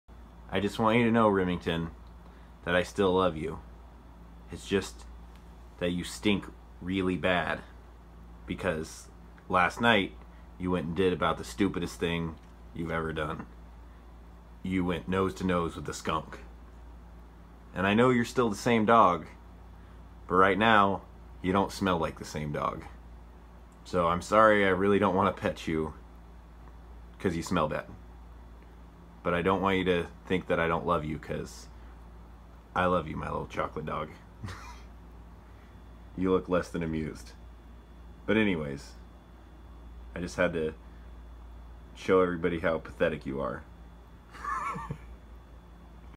Skunked chocolate lab
Dog got into it with a skunk. Me telling him why I can't pet him. He seems to be very upset